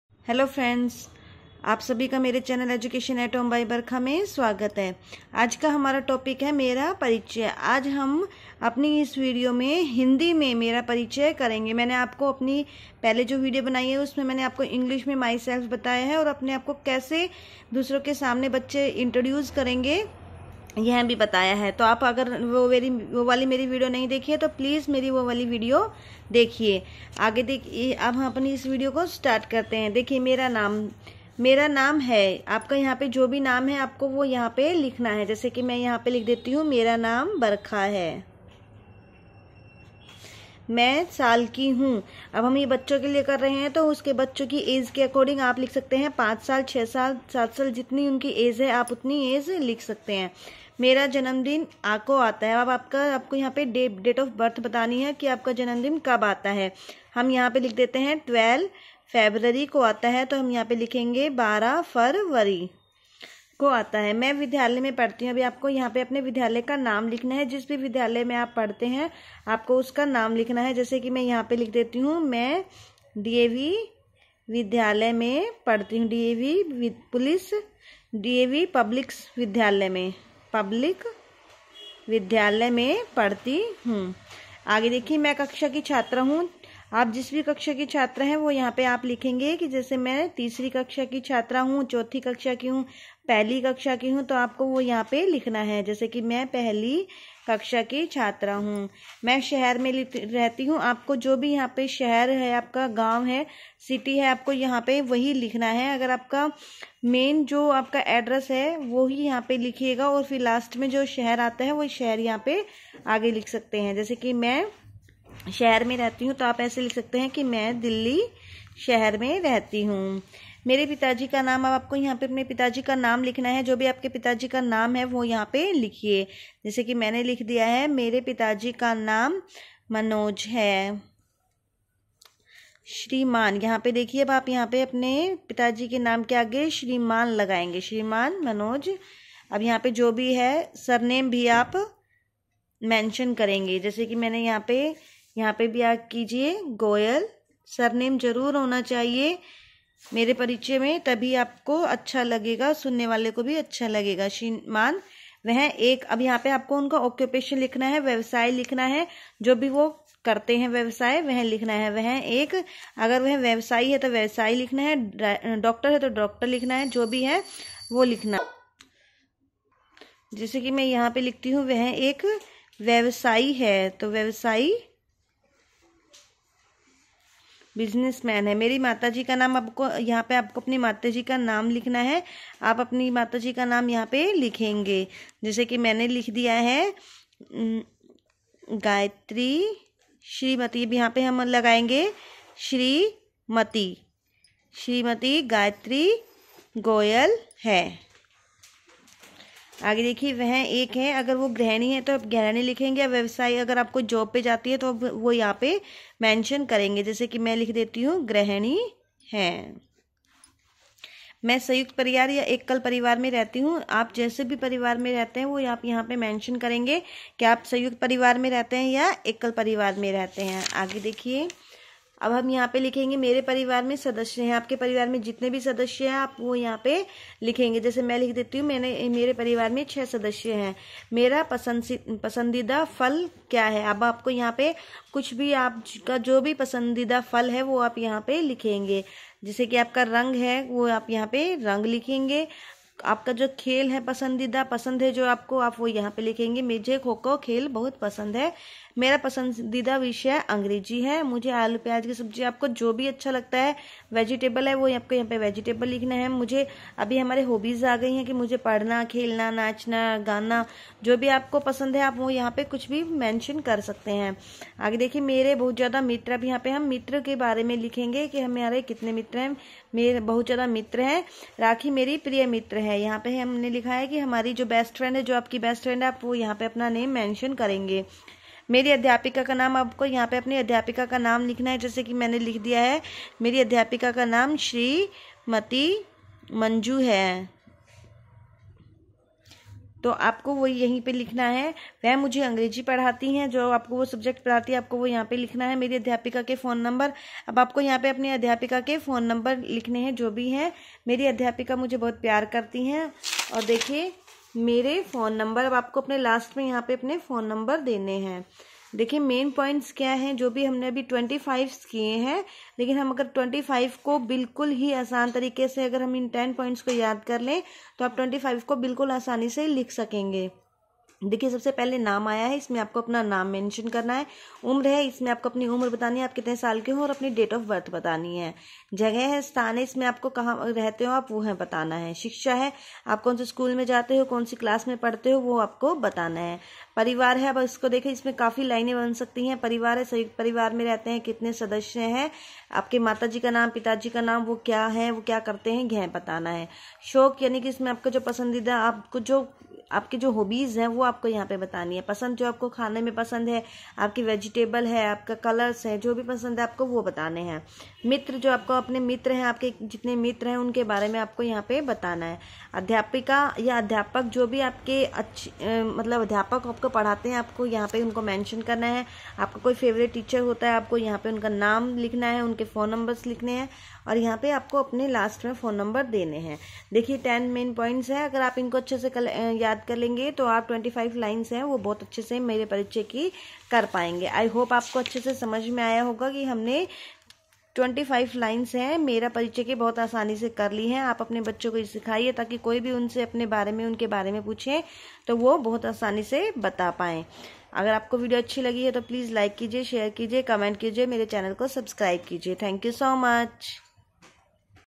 हिन्दी में मेरा परिचय की 25 lines / Mera parichay/ Hindi me Mera parichay
hlo friends in this video we will learn 25 lines of मेरा परिचय it is very easily nd simple to learn and write thanks
mera parichay
Hindi me Mera parichay
Mera parichay Hindi me
mera parichay par 25 lines